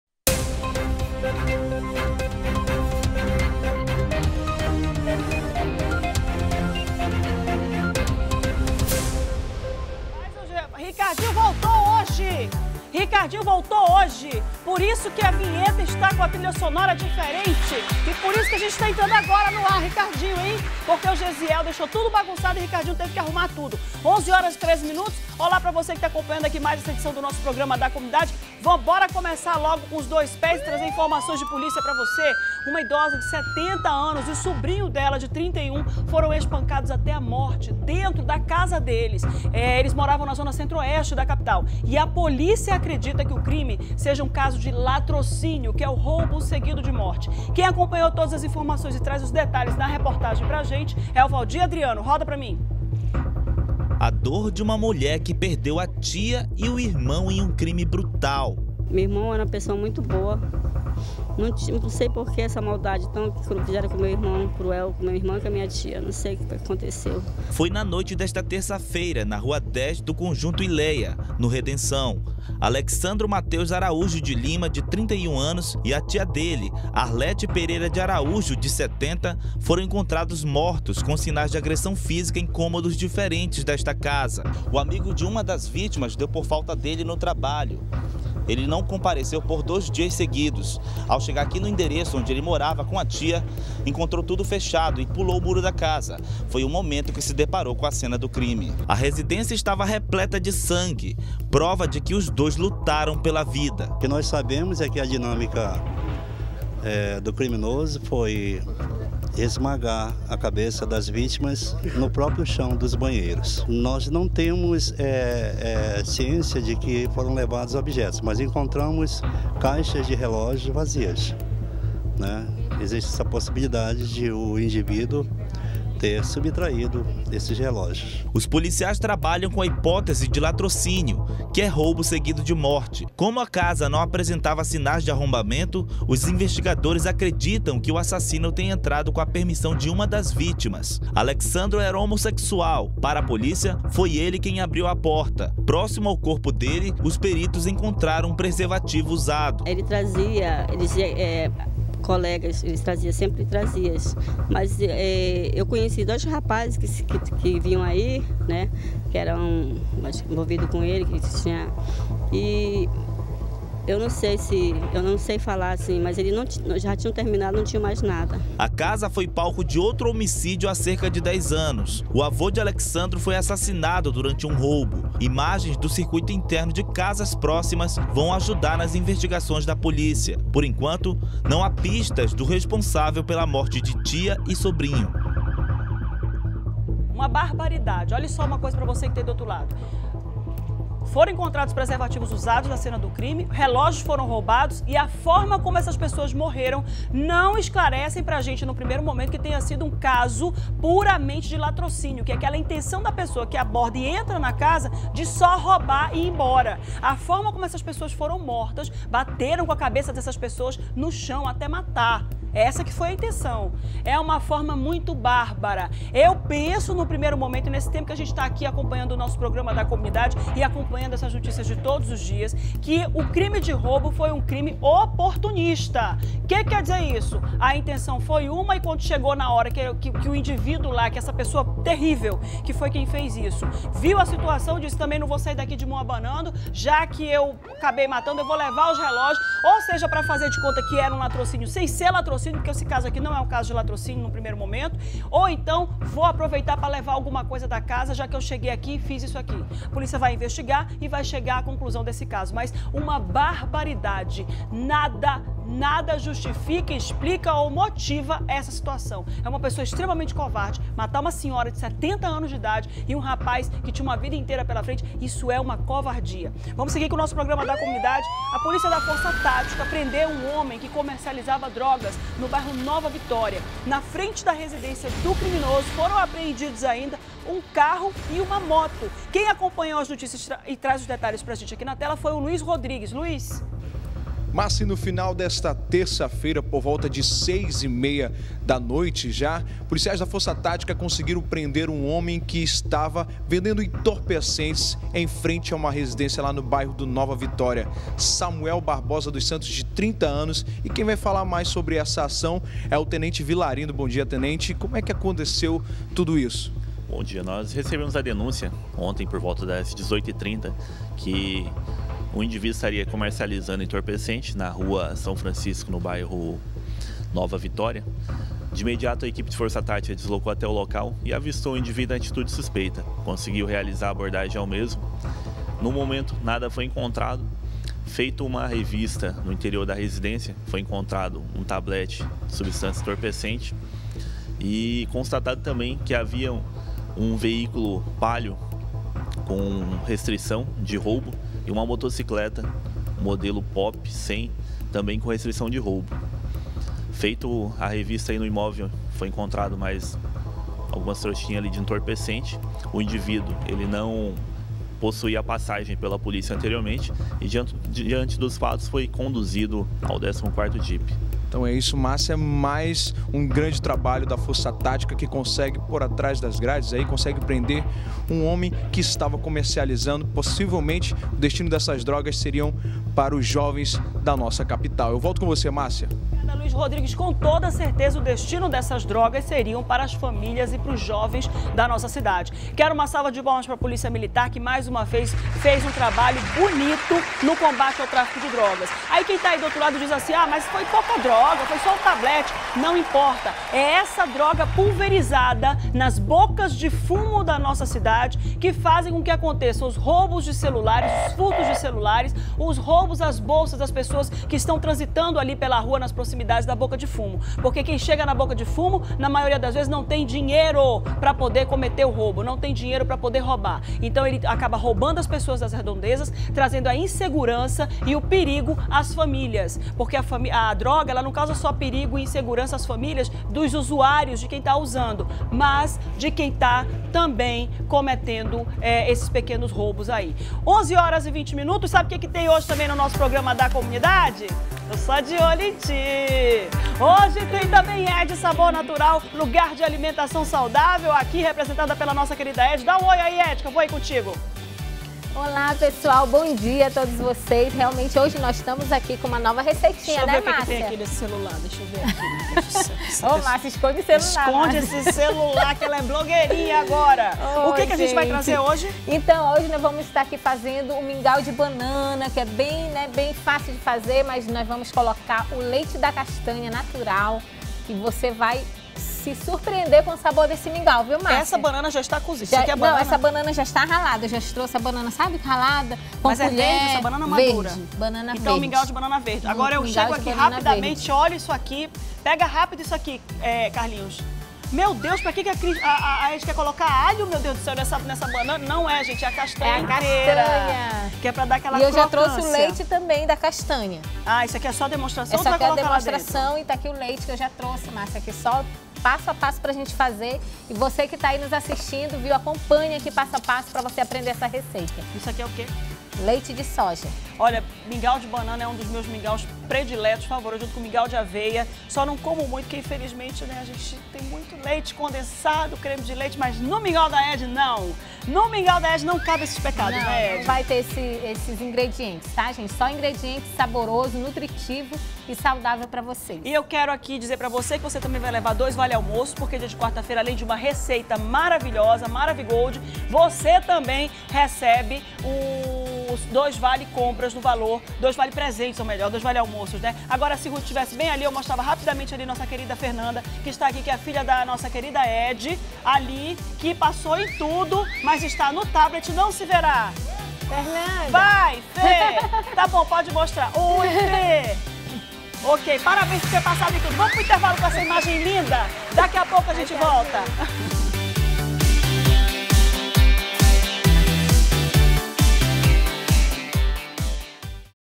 Idosa e sobrinho são espancados até a morte dentro da própria casa no conjunto Hiléia
01º BLOCO AGORA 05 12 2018 - Idosa e sobrinho são espancados até a morte dentro da própria casa no conjunto Hiléia